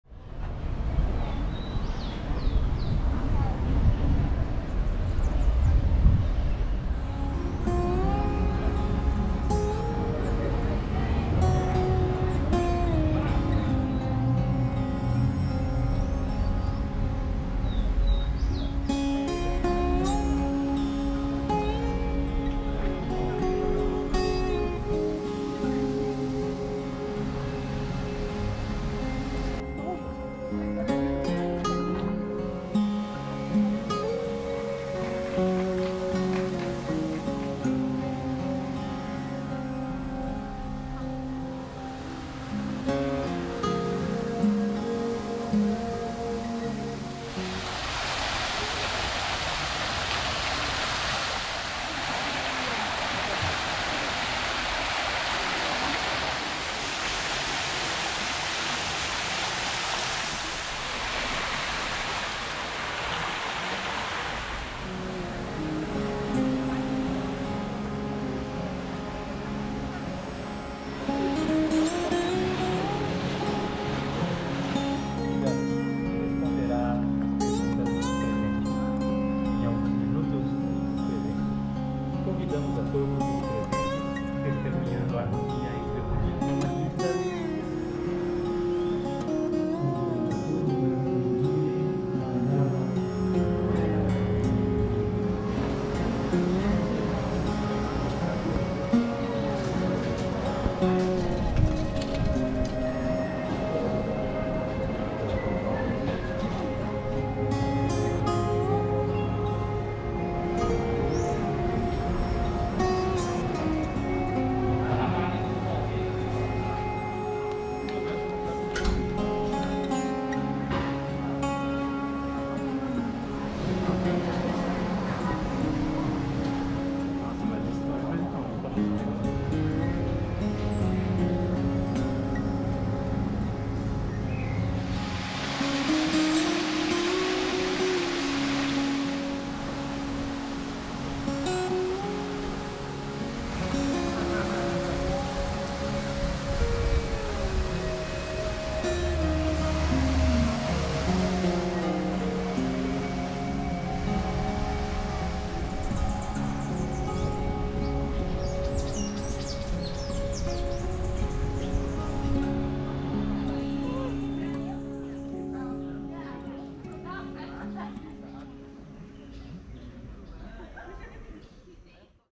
Templo Zu Lai: O Maior Templo Budista da América Latina | Cotia - SP
O templo Zu Lai é o maior templo budista da América Latina, localizada na cidade de Cotia, região metropolitana de São Paulo. A estrutura principal é construído nos moldes da Dinastia Tang, cercado de belos jardins e um lago com uma pequena ponte.
A entrada no templo é gratuita e possui áreas de serviços como cafeteria, restaurante (vegetariano) e uma lojinha de artigos budistas.